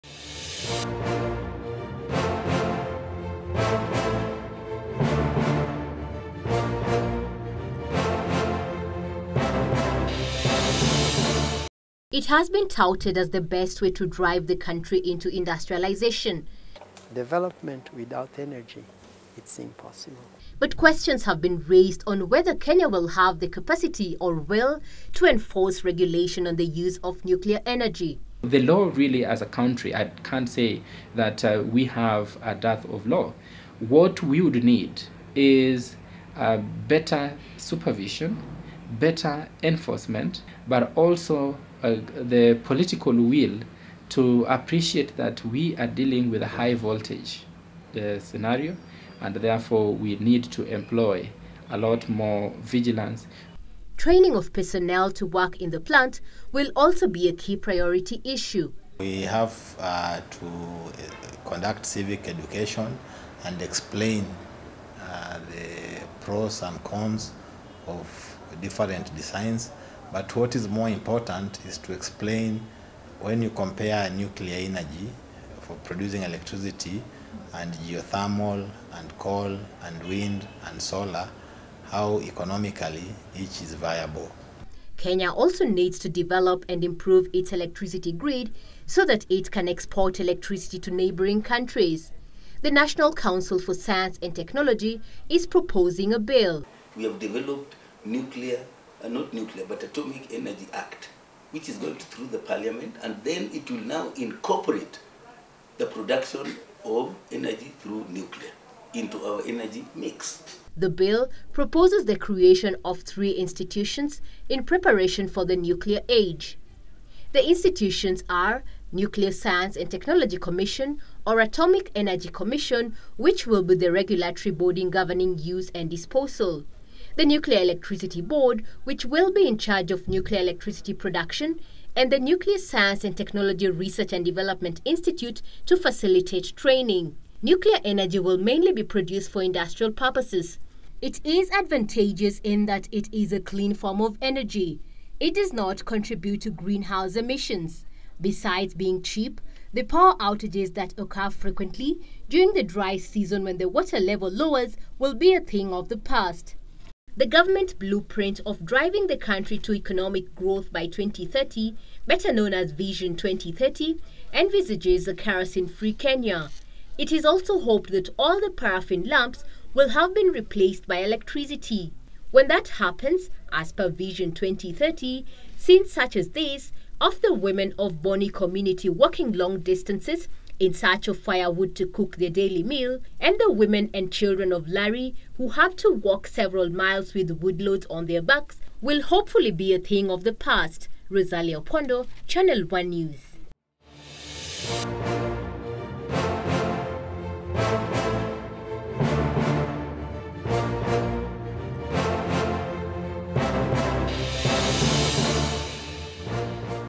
Kenya`s Nuclear energy series 4.
Kenya is striving to become a middle-income Country by 2030, and experts reiterate that a steady, reliable power source is vital. Already there
are plans to table in Parliament, the Atomic Energy  Act, which proposes the establishment of  institutions to manage nuclear electricity production
in Kenya. In the final episode of the series "Tapping the Nuclear Energy Potential," Rosalia Omungo Opondo  takes a look at the road ahead.